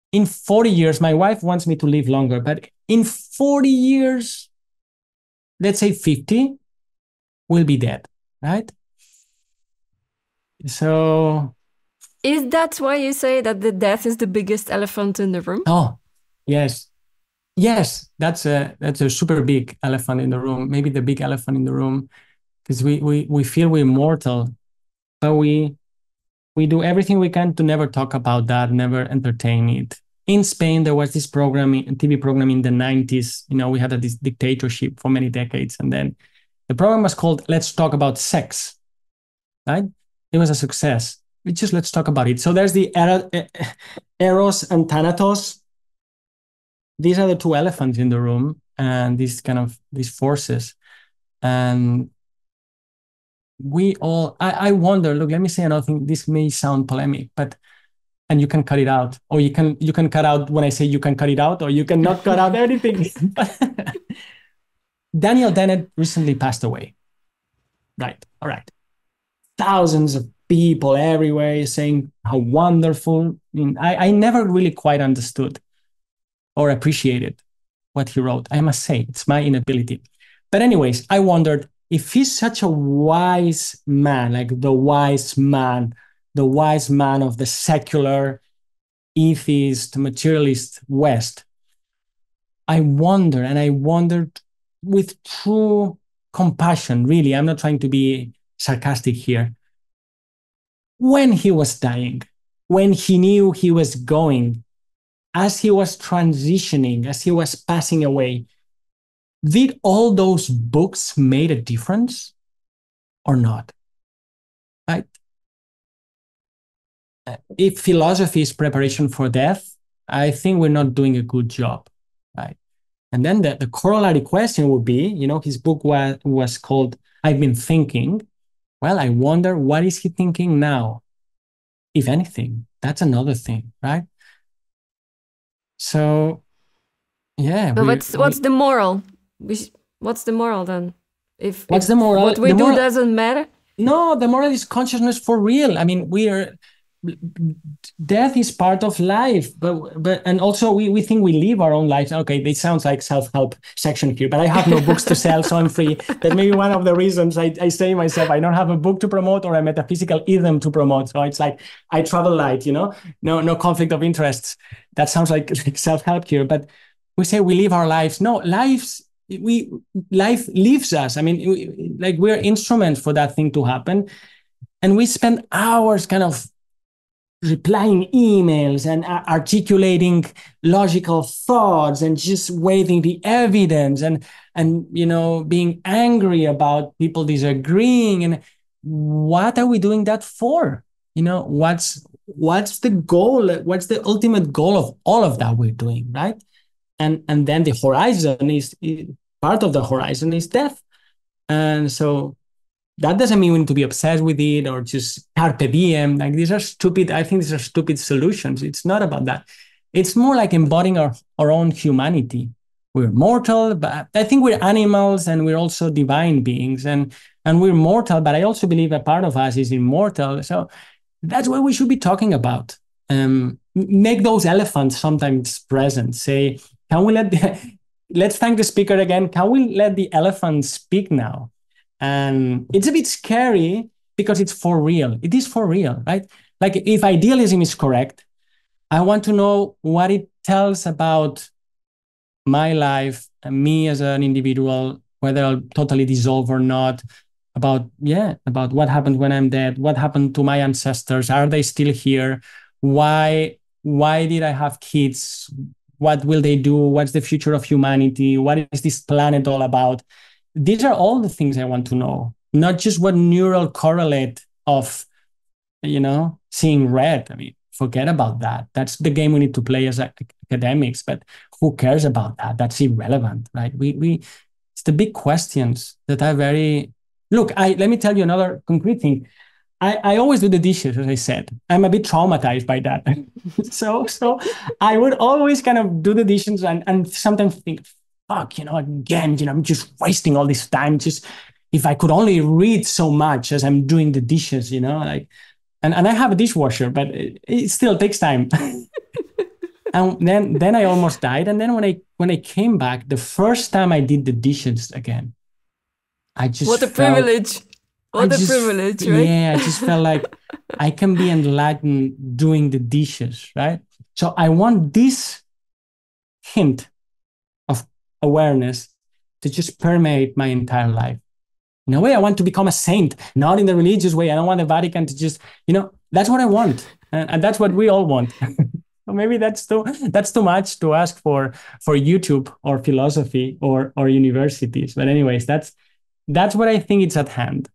Death: A Big Taboo In Science ? | Dr. Alex Gomez Marin
Each week, the Essentia Foundation shares highlights from the most insightful moments of longer videos on this channel. In this video Dr. Donna Thomas describes her own near death experience. In this video Dr. Alex Gomez Marin talks about the fact that science avoids looking at death in a serious way, for instance could it be that part of us is immortal?